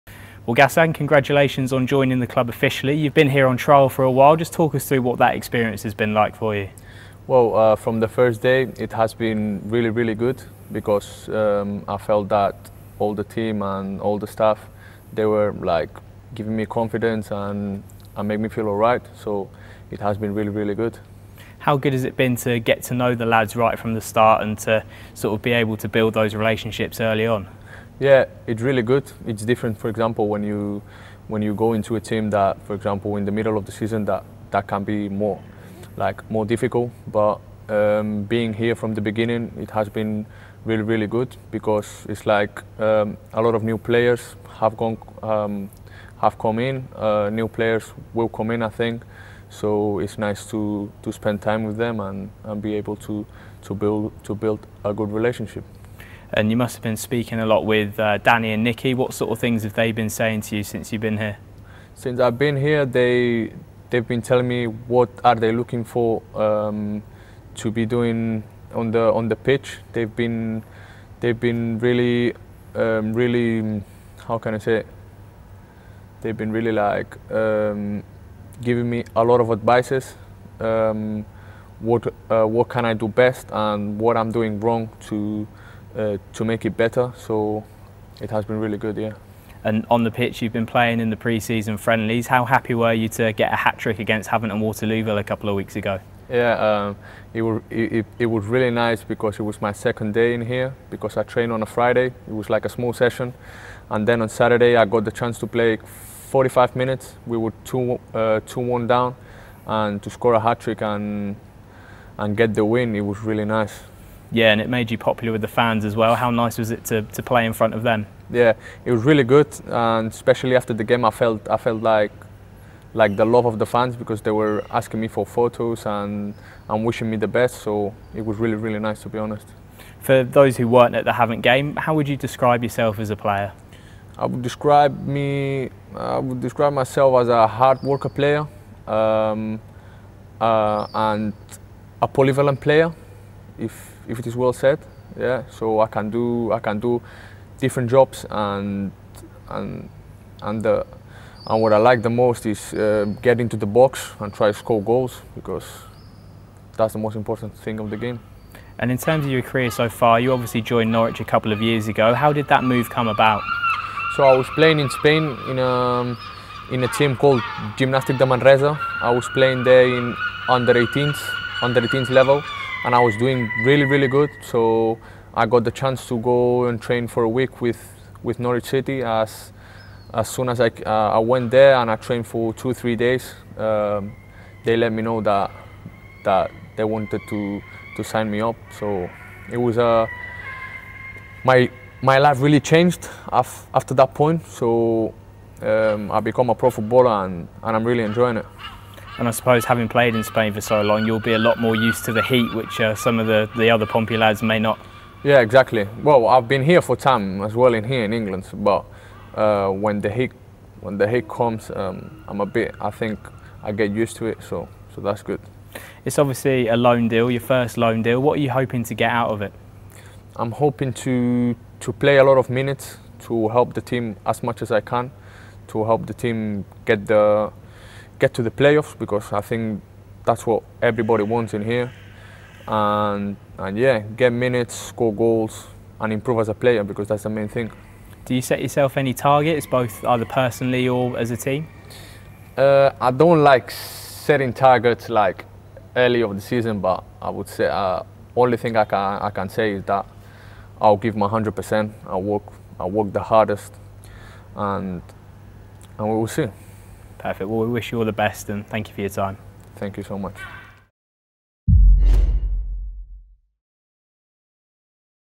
"I'll give 100% and work the hardest" | Gassan Ahadme's first Pompey interview
Forward Gassan Ahadme chats after signing on loan from Norwich City for the 2021/22 season.